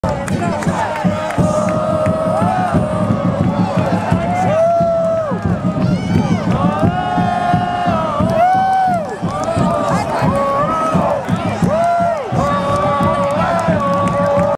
A little of Cyrus Saydee's magic
Detroit City FC vs Minneapolis City SC
NPSL Midwest Region Semi-Final
July 19, 2019
Keyworth Stadium, Hamtramck, MI